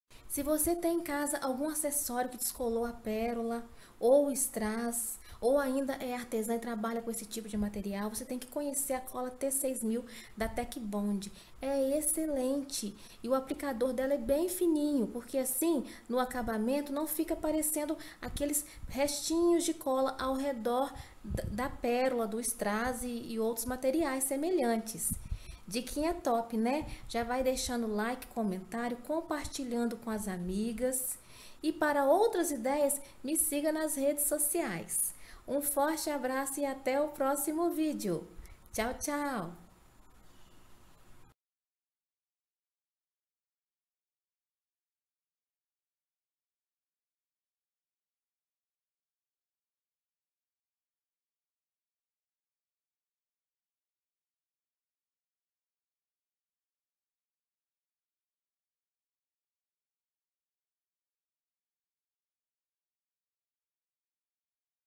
Como colar PÉROLA e STRASS? Dica fácil e rápida - Flávia Martins
Para este passo a passo utilizei:
Cola T6000 da TEKBOND